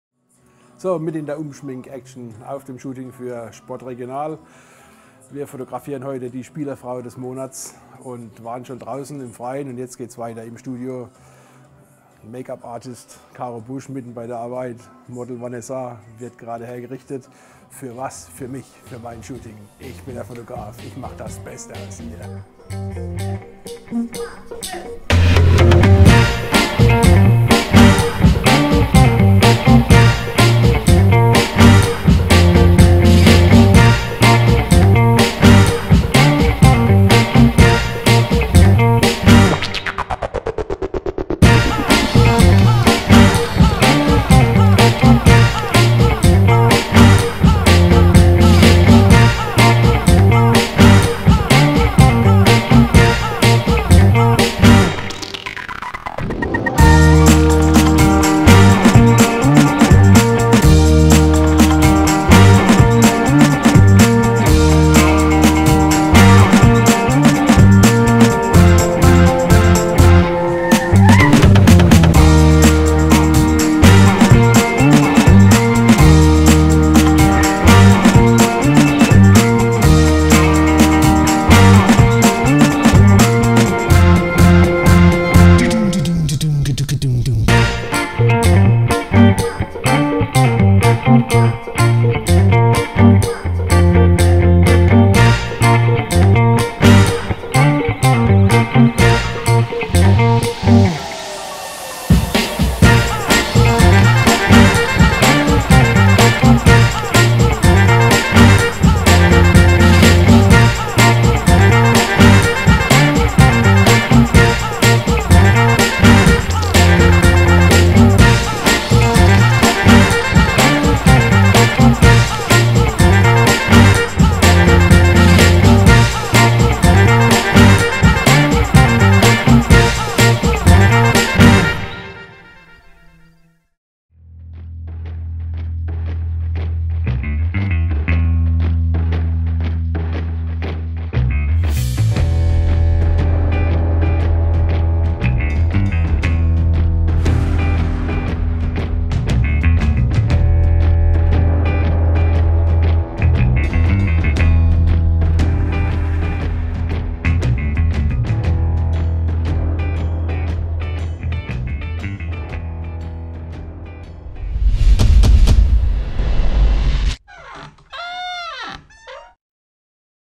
Making of - Spielerfrau des Monats
Unser Making of Video zum Shooting der Spielerfrau des Monats für FuPa.net